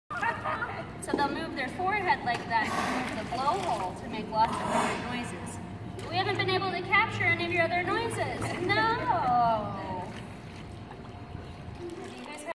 Brooke interacting with a beluga
SeaWorld 12/15/16